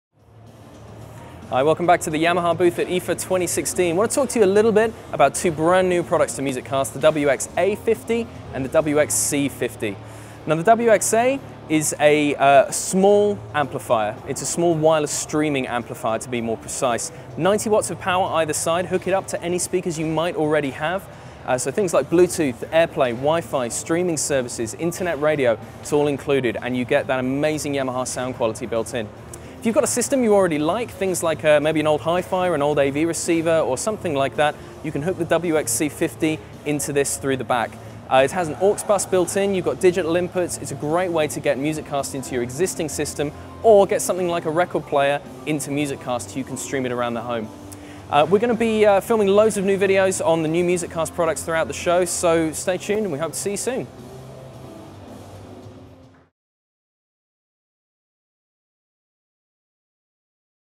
Yamaha MusicCast WXA-50 / WXC-50 / IFA 2016
Simple and Flexible, for Ultimate Listening Freedom

The WXA-50 is a new-concept audio component that goes beyond conventional audio thinking. It let you access a wide variety of audio content such as music from streaming services or stored on a smartphone, and enjoy high quality sound and style thanks to the ability to network with Yamaha’s exclusive MusicCast multi-room audio system. High functionality and performance bring you musical enjoyment just the way you want it.

MusicCast is a new revolution in network audio, use all your MusicCast products together or separately, it’s completely up to you. Control it, all from one app, designed to be intuitive, quick and simple to use. Use a soundbar, a wireless speaker, an AV Receiver, HiFi audio whichever suits you best, mix and match then expand your MusicCast system as time goes on. All new from Yamaha, the MusicCast system brings everything to everywhere for everyone.